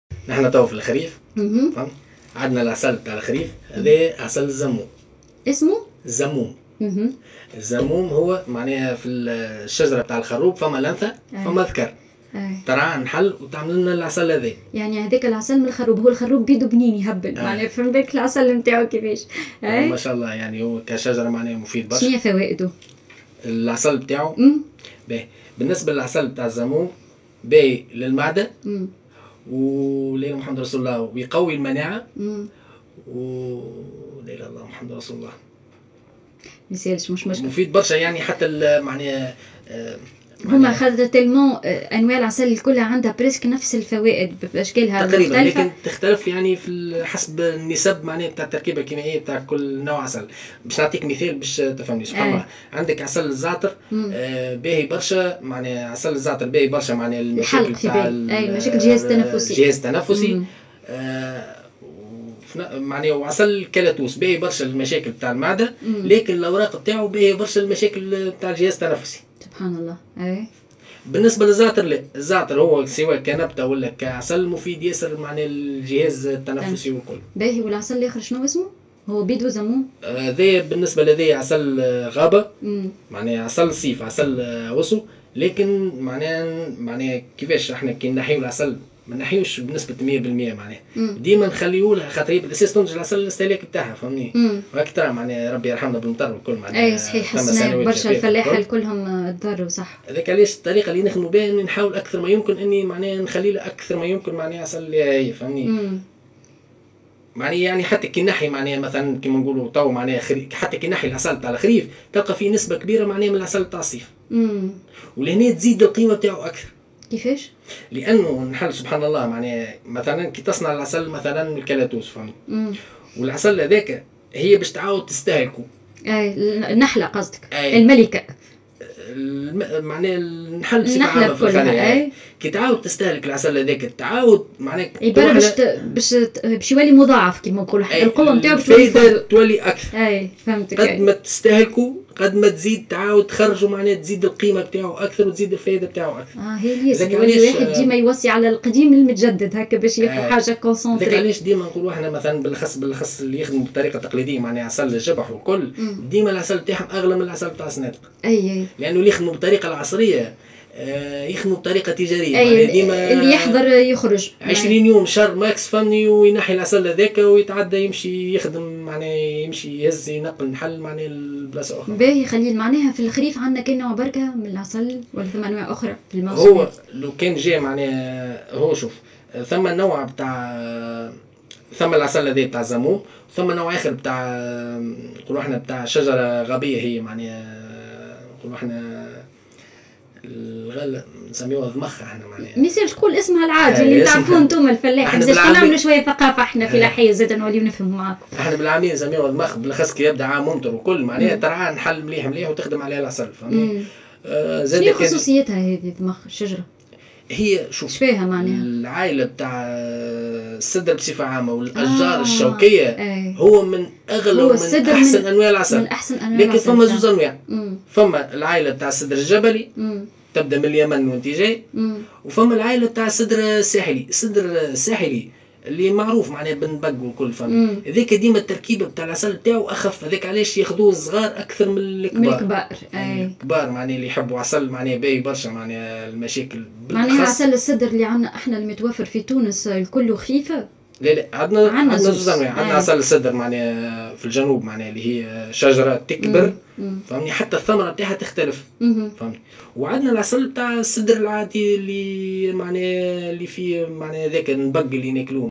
انواع عسل الخريف والفوائد الصحية الخاصة بكل نوع
انواع عسل الخريف والفوائد الصحية الخاصة بكل
نوع